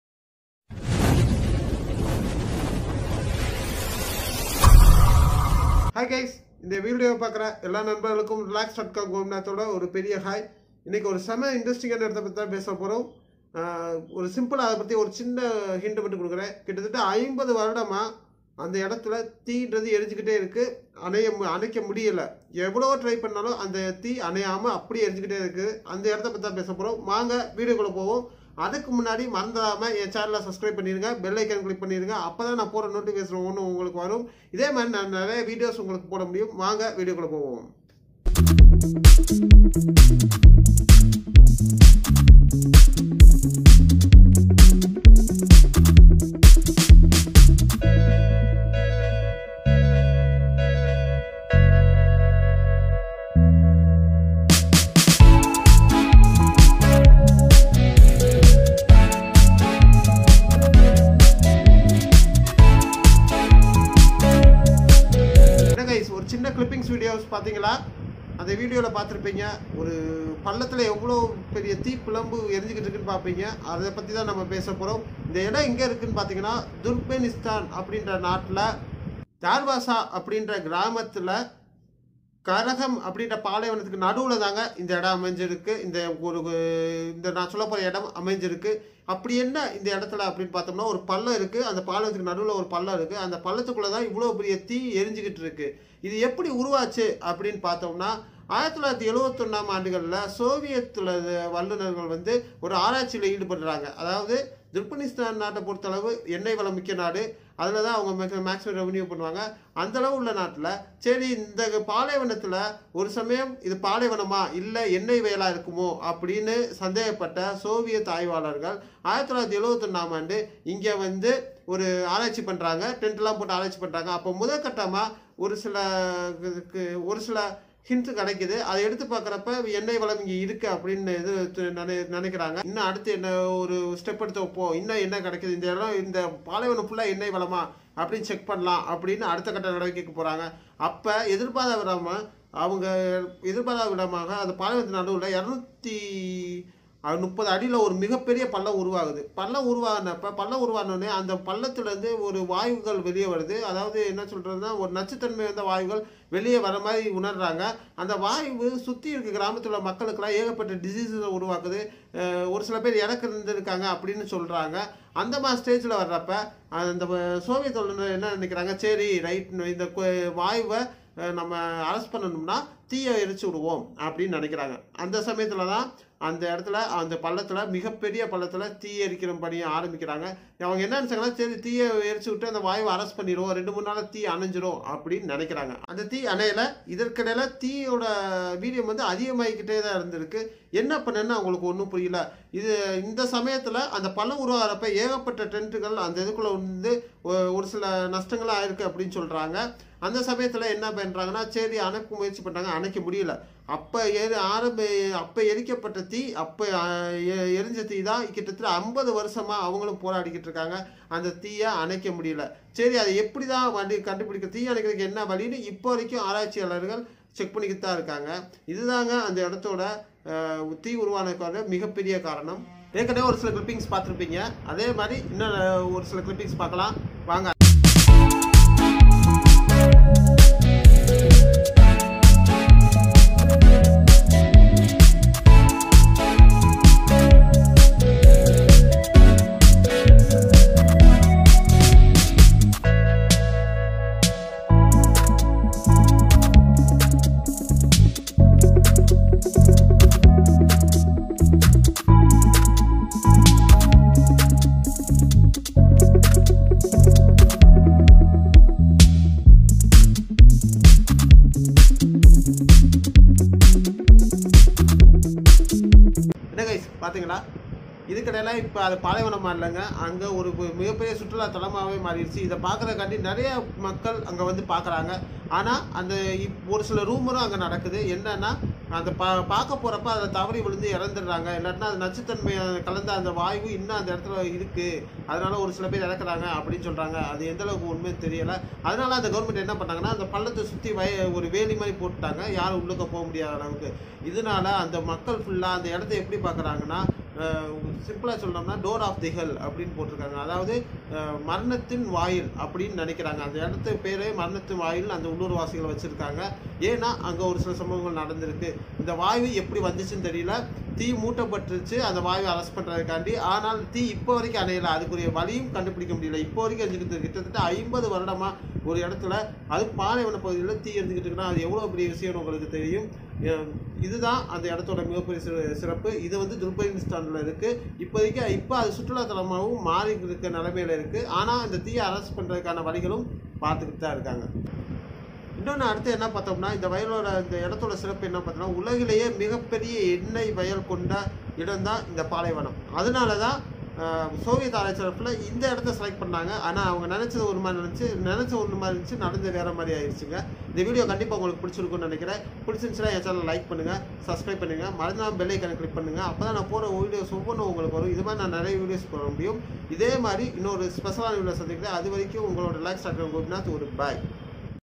Mystery video#Amazing video தொடர்ந்து 50 வருடமாக அணையா நெருப்பு 🔥🔥
This video is explained above 50 years of continuous fire can be explained in this video and the fire how can be Spark in this video.
Fire Mystery video தொடர்ந்து 50 வருடமாக அணையா நெருப்பு 🔥🔥
Mystery video
Amazing earth